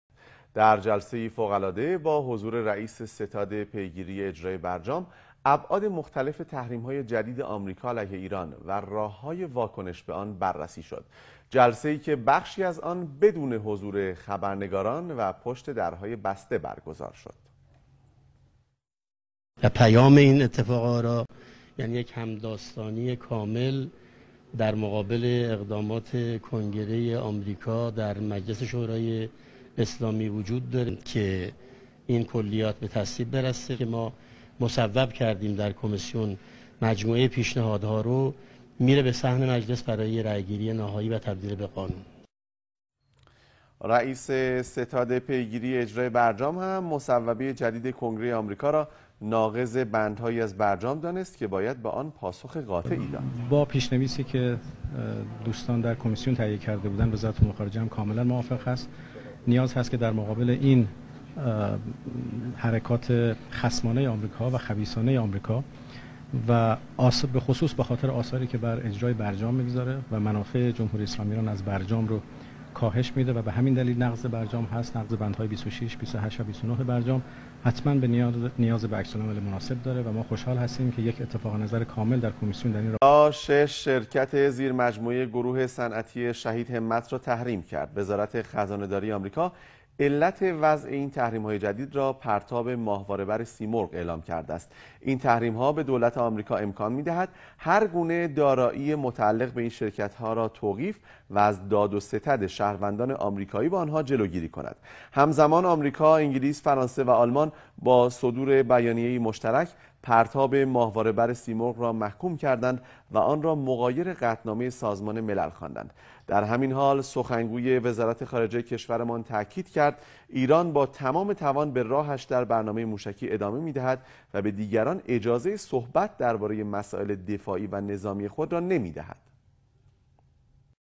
Majlis special committee approved reaction to United States
Majlis special committee approved reaction to United States on Saturday 29-07 which will be voted on fl0or of Majlis on  30 July 2017 .